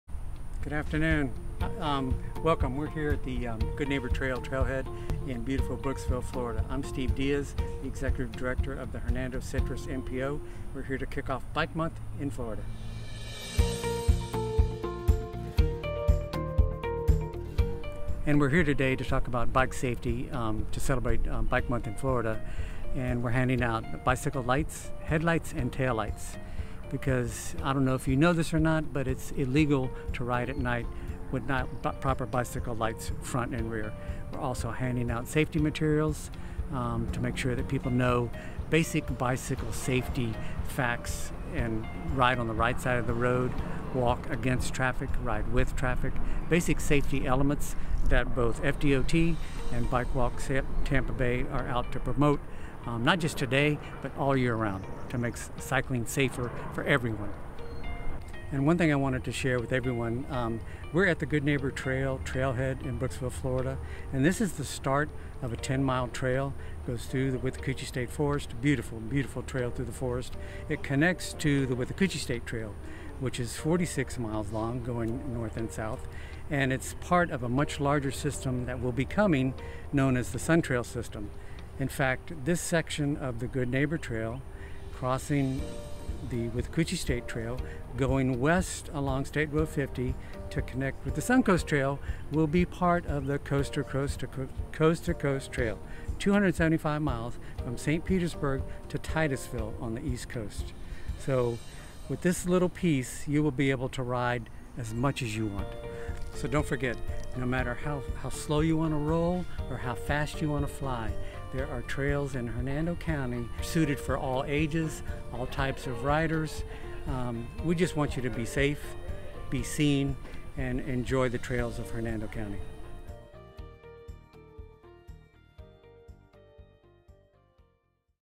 Florida Bike Month in Hernando County (2021)
A video I created for Florida Bike Month. Special thanks to Steve Diez for his help.

Filmed Feb. 26, 2021.